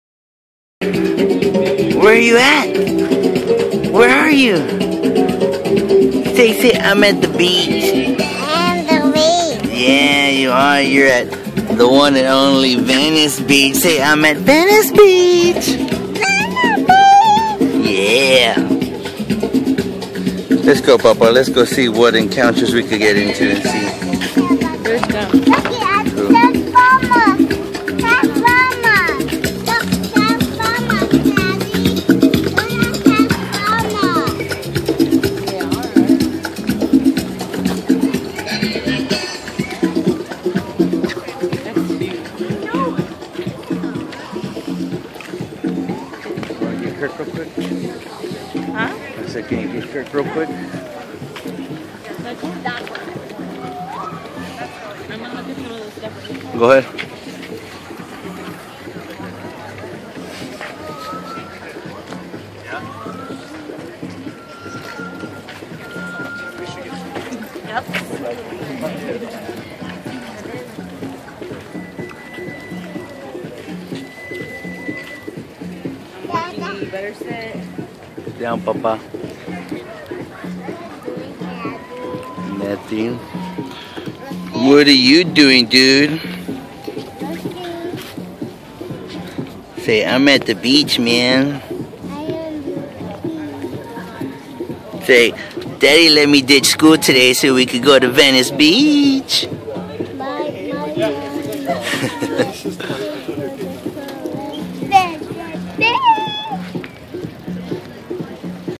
KIRK at Venice Beach, California 1-15-2014
Took the day off from work to take Birthday Girl Arely and our little champion Kirk to the one and fucking only Venice Beach, California on Wednesday January 15, 2014